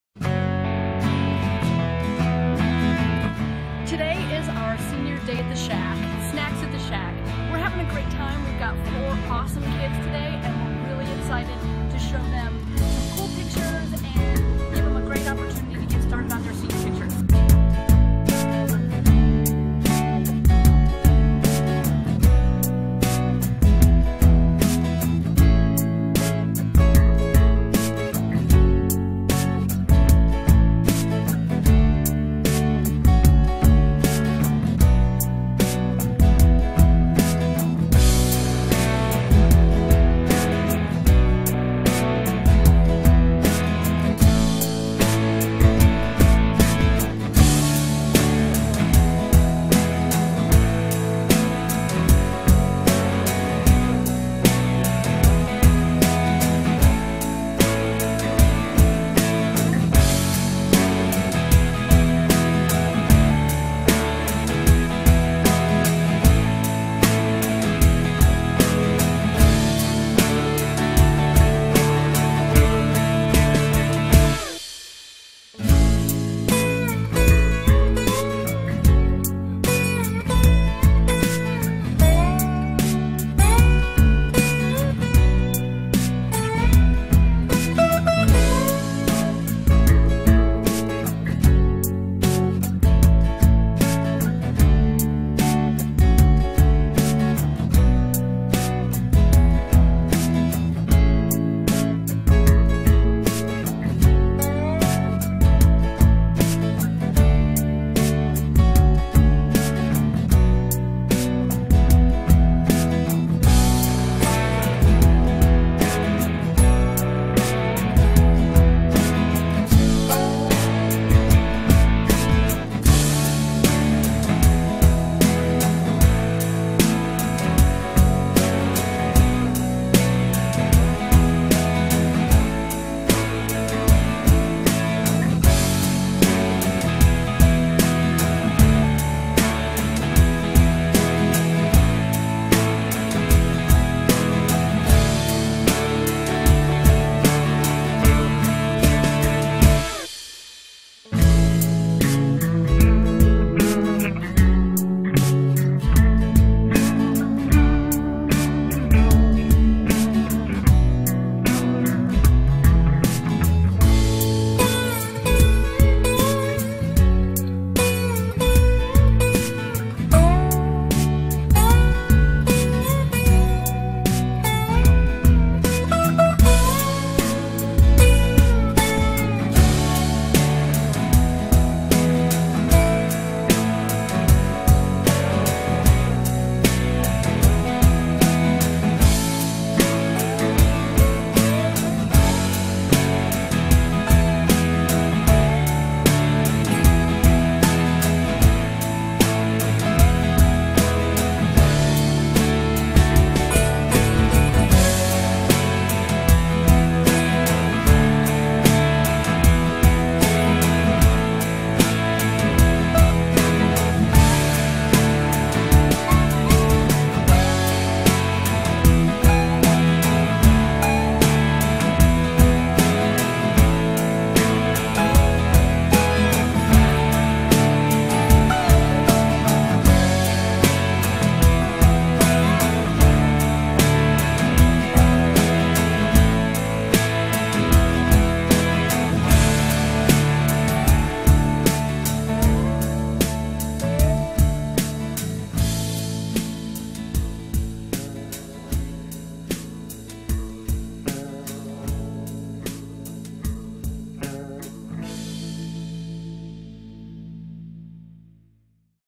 Snacks at The Shack with Kat Mack Photography
Kat Mack Photography hosted a special event for our VIP Seniors.  The class of 2017 incoming  seniors are the select group that wanted to be the face of KMP and will be a part of our advertising and promotions for the new Senior High School Season.